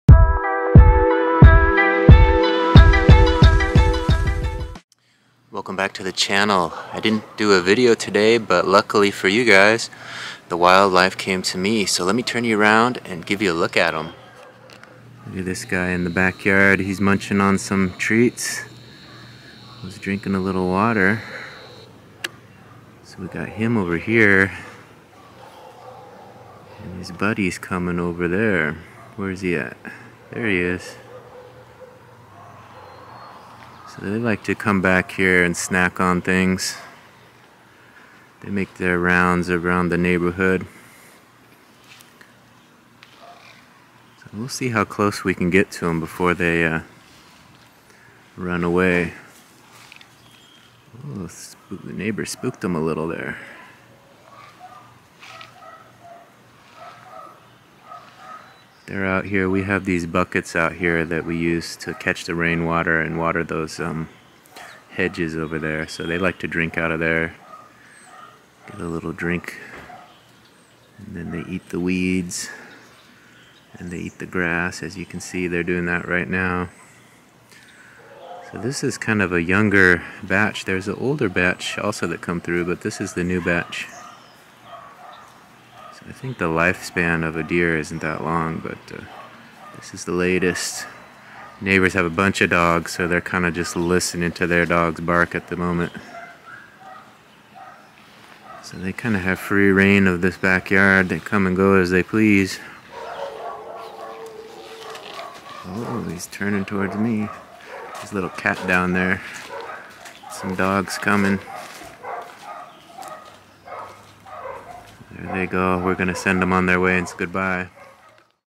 Unexpected backyard visitors surprise me by walking on in and helping themselves to a meal and drink
In today's video, a couple backyard visitors walk into my backyard and just help themselves to a meal and drink.